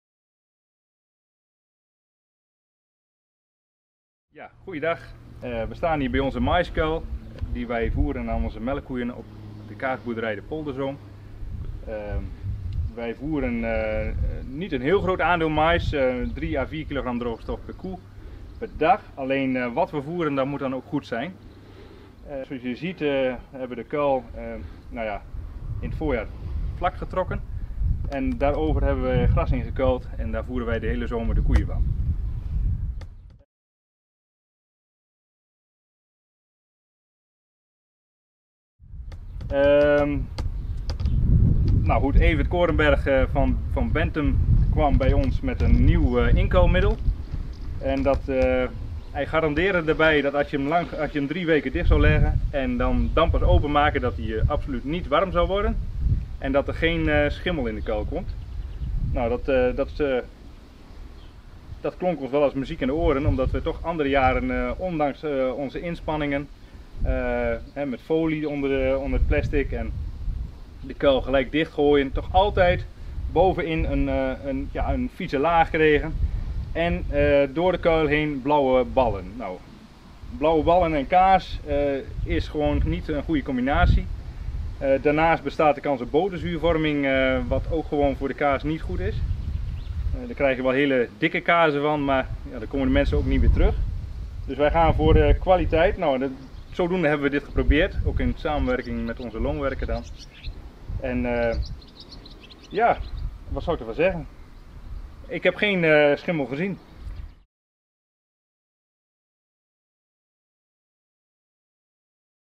Kaasboerderij de Polderzoom en van Benthem diervoeders voor beterruwvoer
Van goed ruwvoer naar beter ruwvoer
Belangrijk dat de dieren het ruwvoer goed vreten , het voer fris blijft en gisten, broei en schimmels niet uitbreken. Boterzuur en kaas produceren gaat ook niet samen. Smakelijk en gezond ruwvoer is ook bepalend voor gezonde koeien en een gezond rendement.
Magniva Platinum 1 voor snijmais draagt hier enorm aan bij